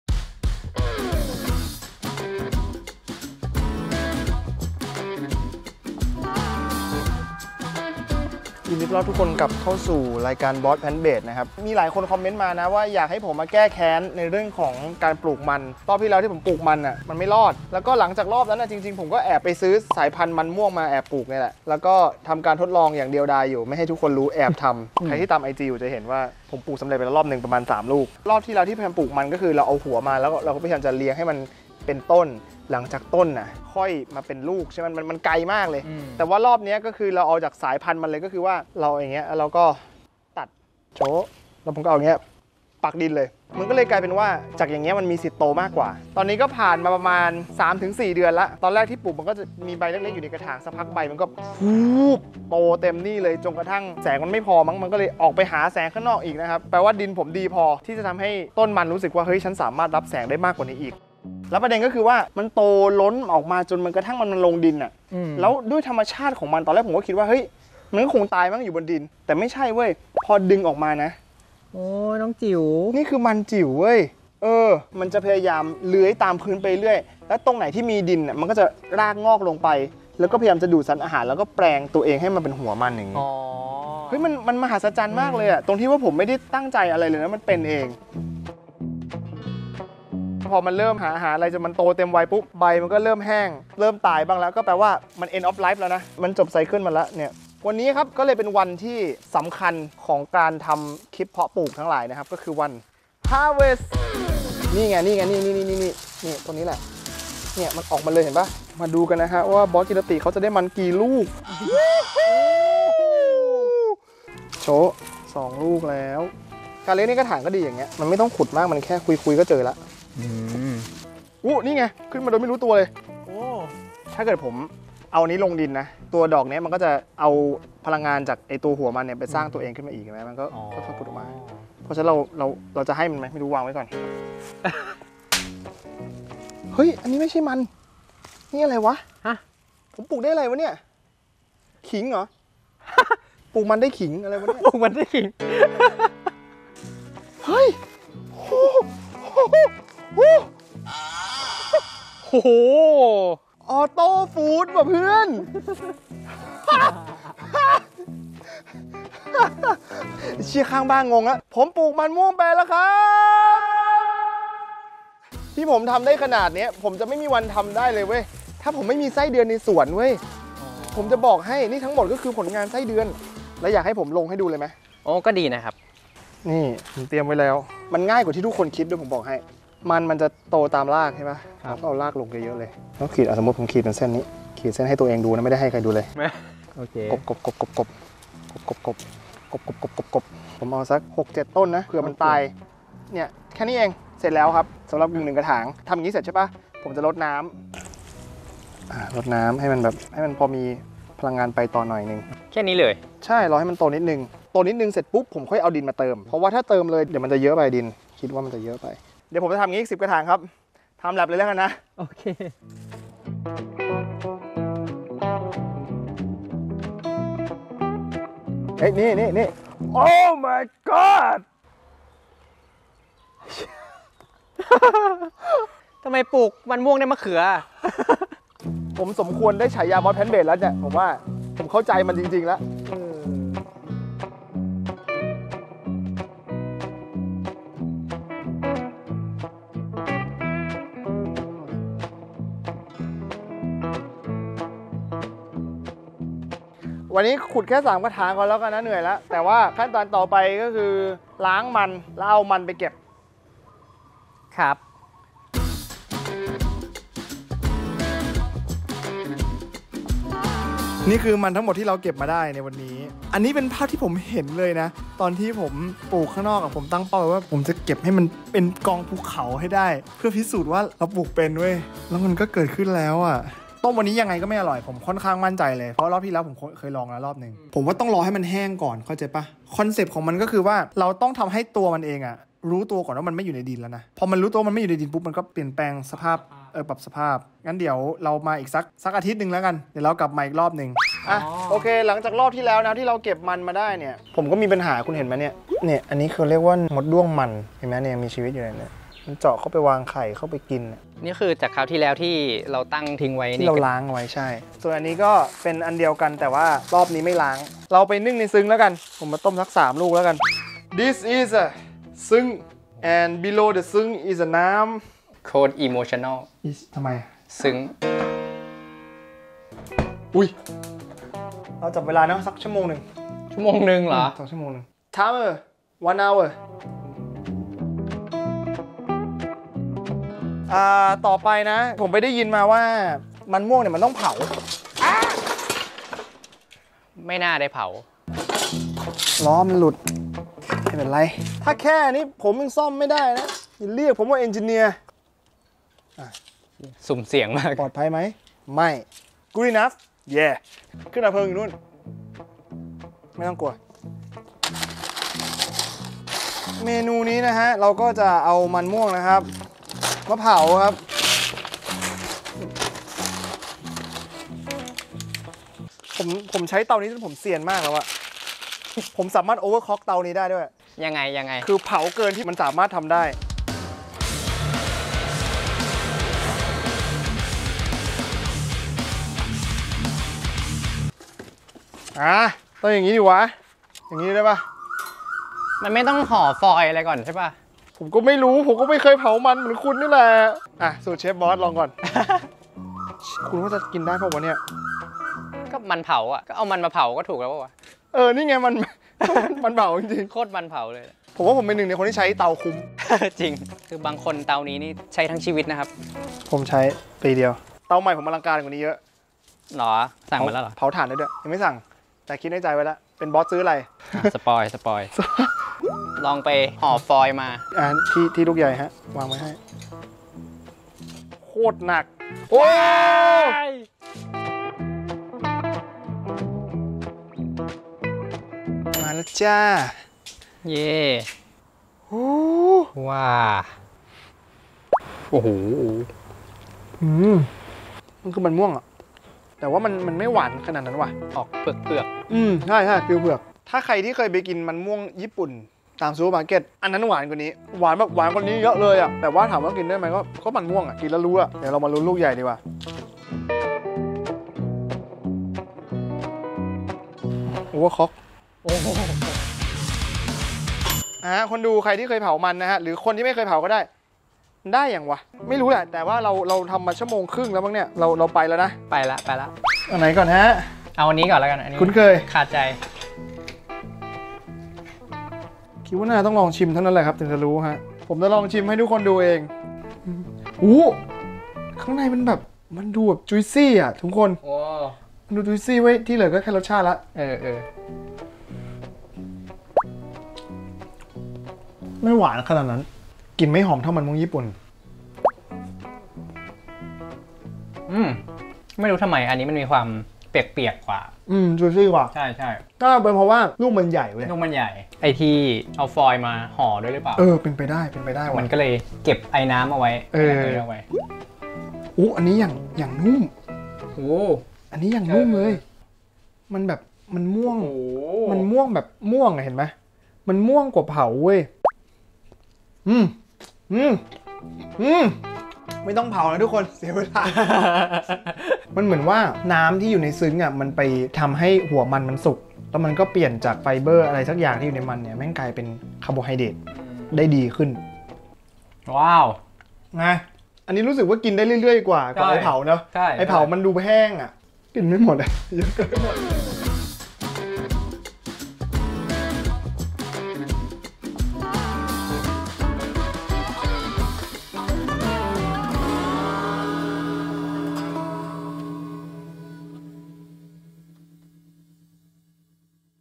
🍠 ปลูกมันม่วงเป็นแล้วครับ! 🍠 | บอสแพลนท์เบส Ep.11
ลองมาเป็นปี ในที่สุดก็มีมันม่วงกินตลอดปีตลอดไปแล้ว 🍠 🍠 🍠

สามารถติดต่อผมได้ผ่านช่องทางต่อไปนี้ค­รับ